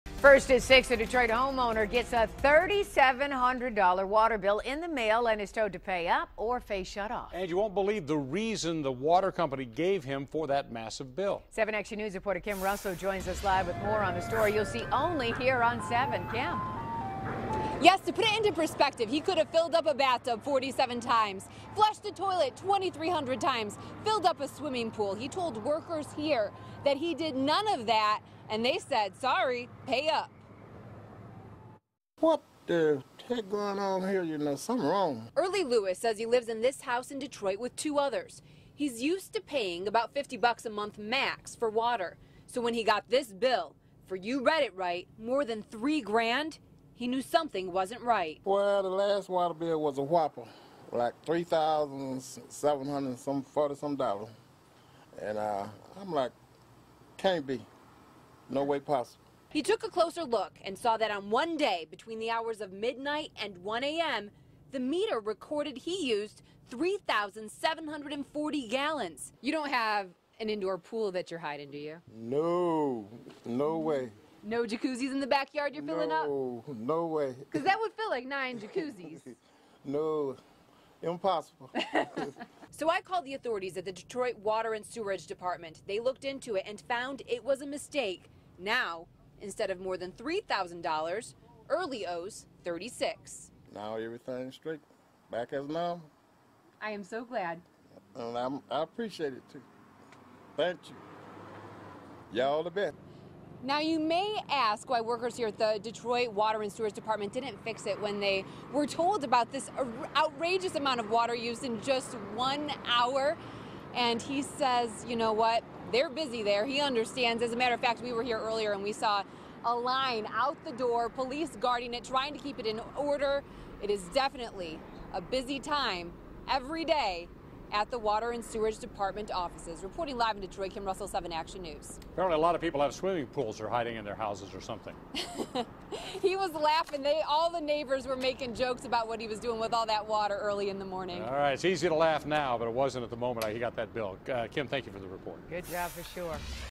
Detroit homeowner receives $3,700 water bill
A Detroit homeowner was shocked when he opened his mail to a $3,700 water bill from Detroit.